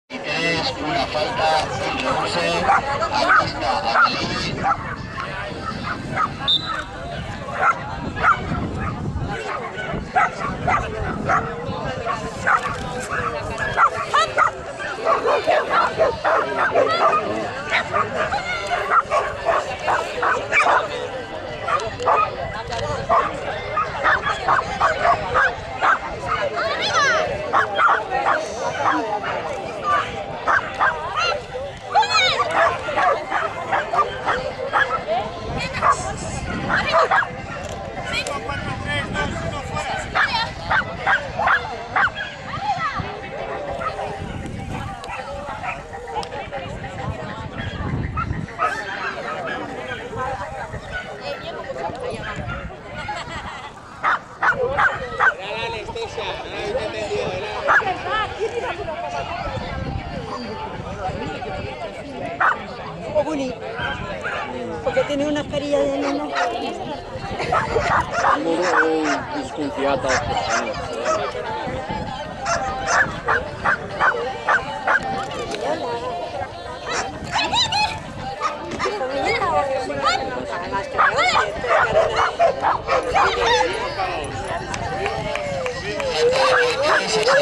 Agility 26.03.16 Amelie westie
2º pista de mi chica, conseguimos 1 punto y 1º puesto podium!